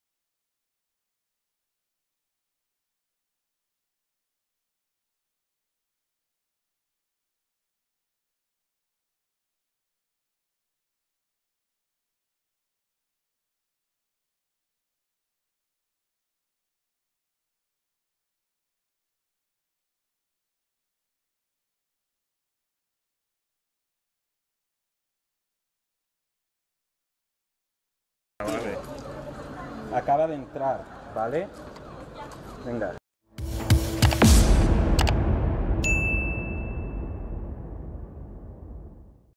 La Policía busca al conductor huido que mató a dos personas mientras escapaba de la Guardia Civil
La Policía busca por todo Madrid a un hombre fugado del escenario donde este mediodía fallecieron dos personas y varias más resultaron heridas tras ser atropelladas por un vehículo que circulaba a gran velocidad. Responde a las siglas P.V.S., nacido en 1991. El coche, un Mercedes gris, huía de varios coches camuflados de la Guardia Civil que le perseguían desde Leganés tras no atender a una orden de alto. El conductor, tras abandonar el vehículo y a sus otros dos ocupantes -más un bebé-, huyó del lugar a pie.